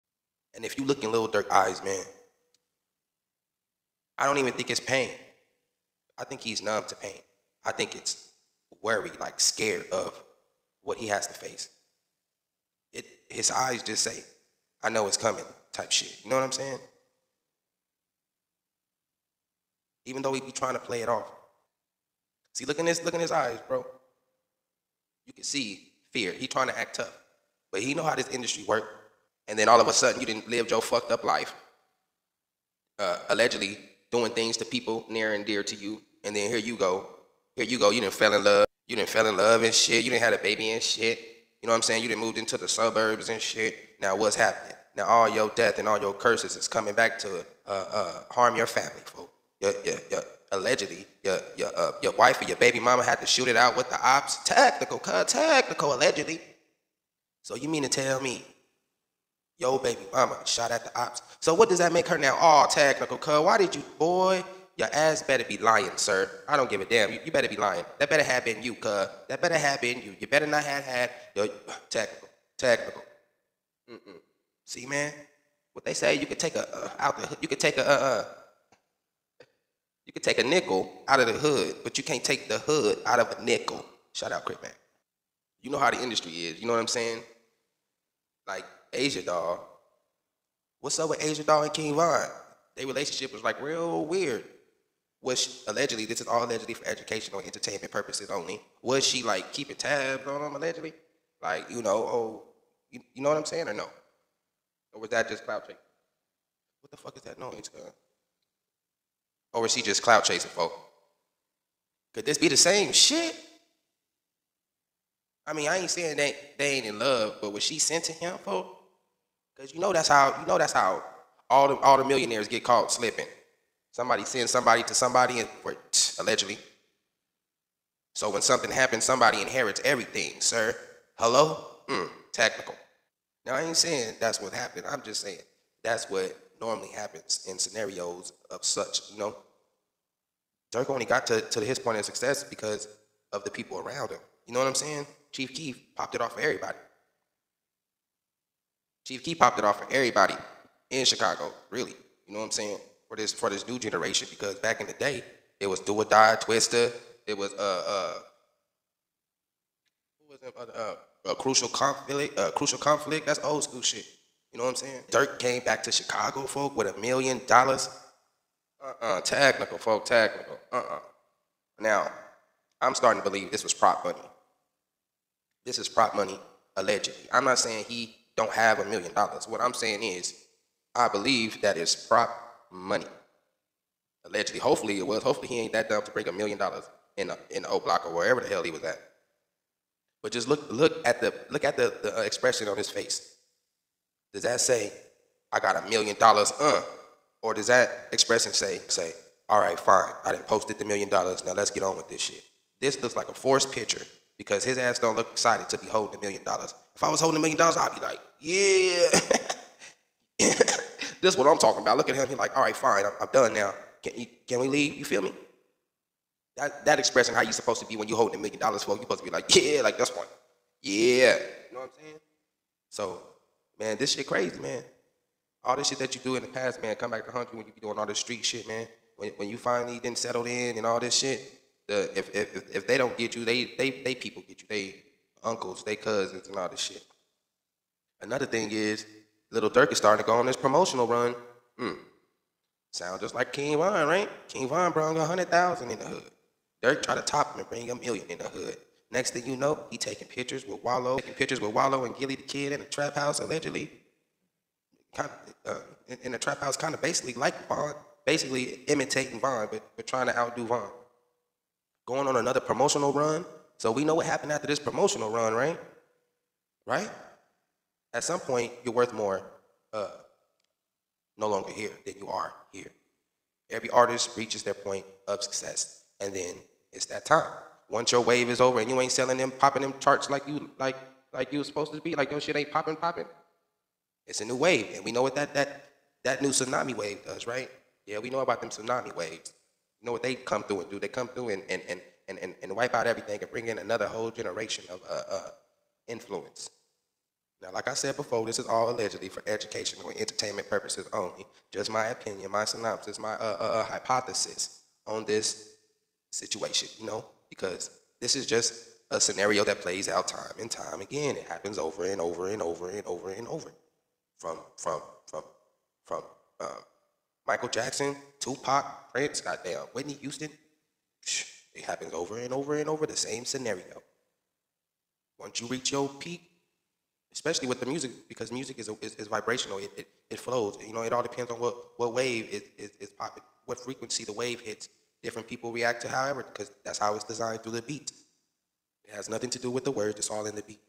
Did Lil Durk know the Feds were coming
Lil Durk Arrested for murder For hirer Lil PaB With otf Dada